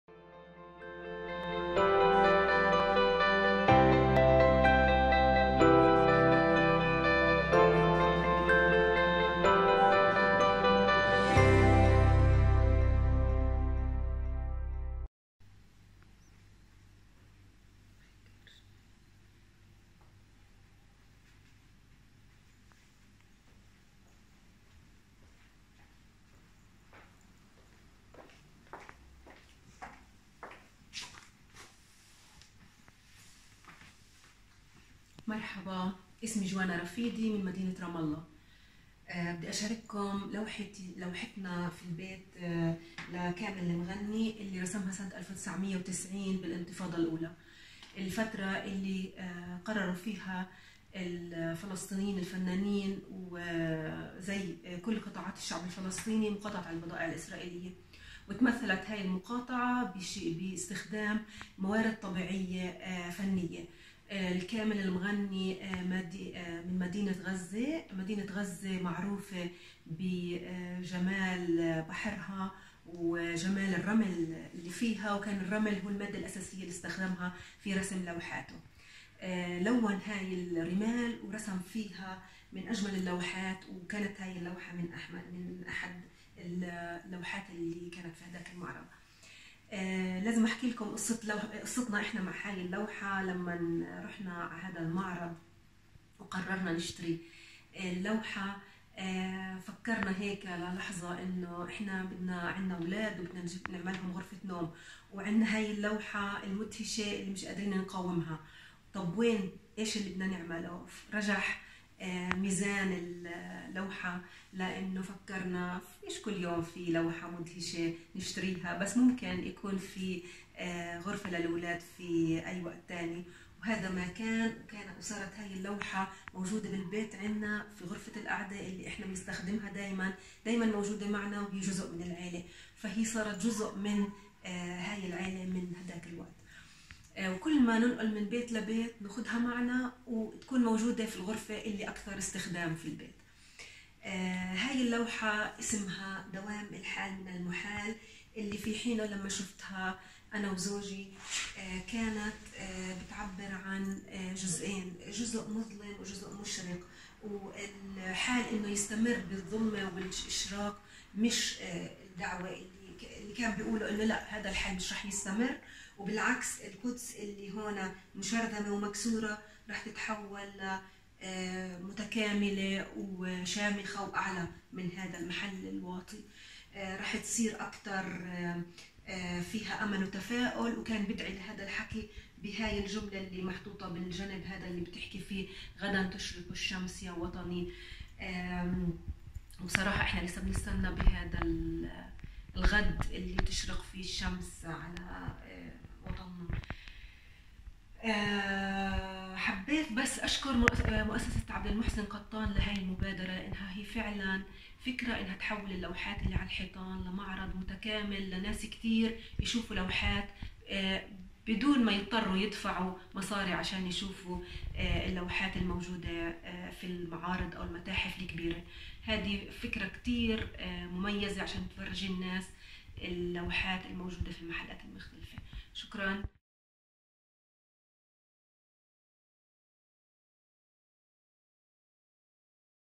جوانا رفيدي - على الحائط
مشروع "على الحائط" يهدف إلى تجميع مشاركات من خلال الفيديو للأشخاص من الأماكن كافة في فلسطين، حول قصص اللوحات الفنية أو الصور العائلية أو معلقات تاريخية أو غيرها من الأشياء المُعلقة على الحائط في بيوتنا، ومعرفة أهمية هذه المعلقات الشخصية أو الفنية أو الاجتماعية أو السياسية للناس، وسرد القصة من ورائها.